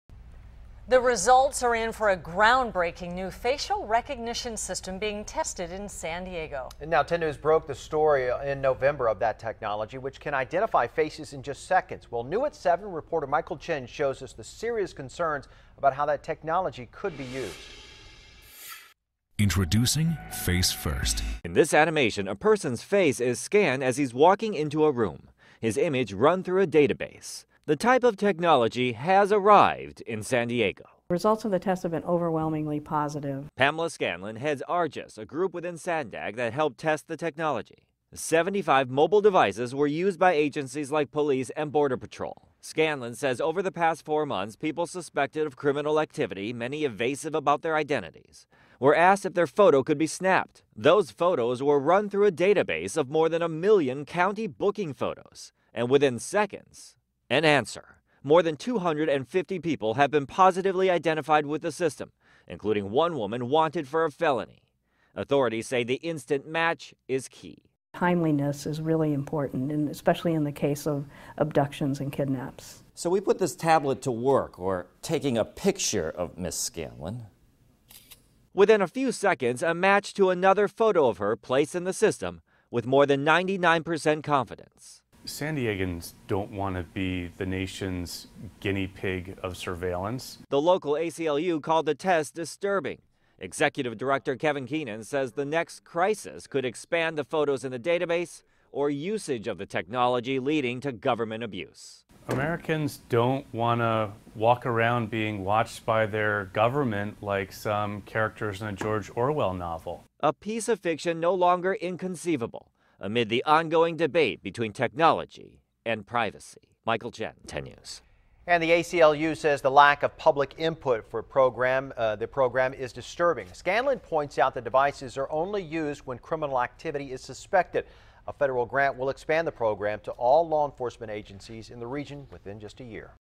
Instant facial recognition now in place for local law enforcement
Results are in for a new, groundbreaking facial recognition system being tested in San Diego.